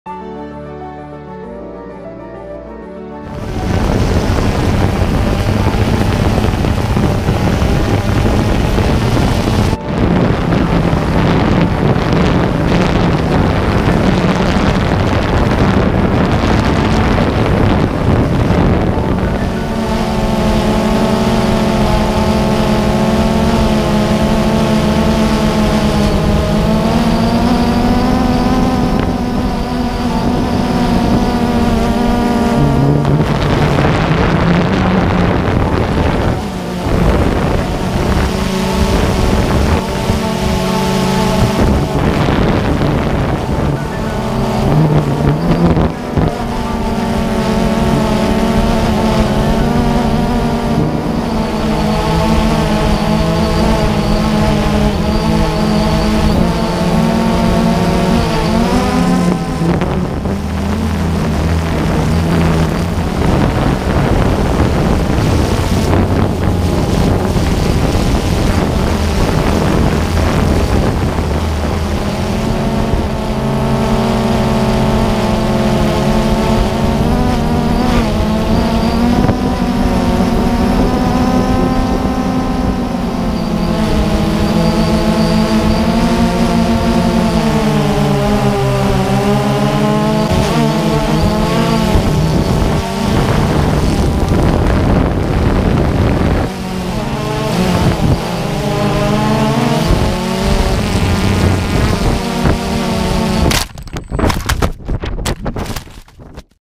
bluefish river Duxbury MA shot in 4k
Rough footage at 4k of early evening flight on Bluefish River in Duxbury, MA